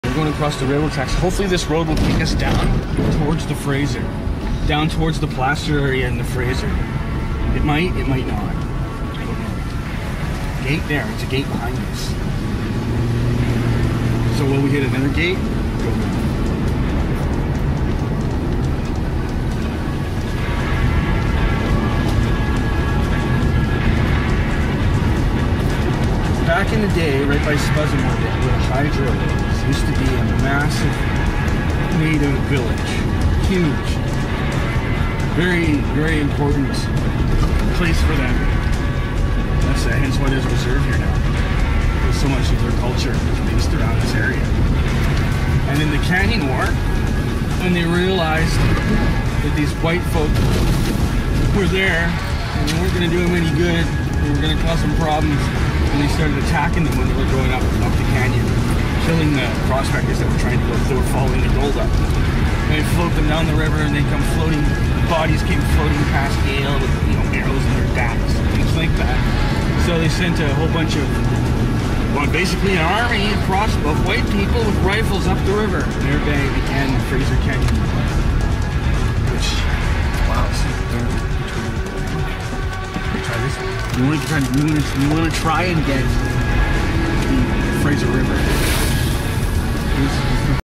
Following the road to Hidden Gems of History in the Fraser Canyon
Embark on a journey to discover hidden gems of history in the Fraser Canyon. Follow the road to power, where Natives found spirits and prospectors found gold. Explore the rich history of this fascinating area!
Journey to the Fraser and learn a little history as we drive on down the road. We will also talk Gold Rush History while on location.